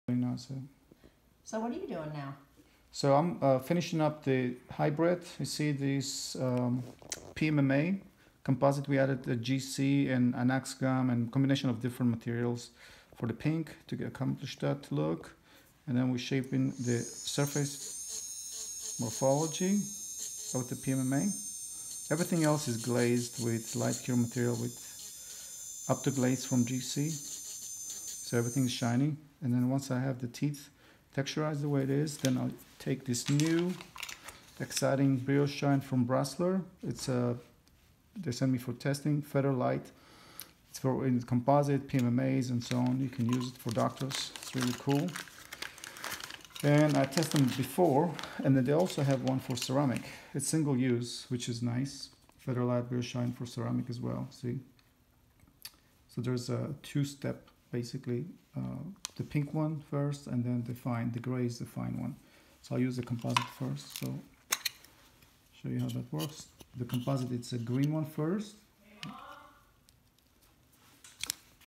Shaping An Implant Retained PMMA Restoration
The way you shape the PMMA (temporary) is just as important as the way you shape the final zirconia.  It is the blueprint for success.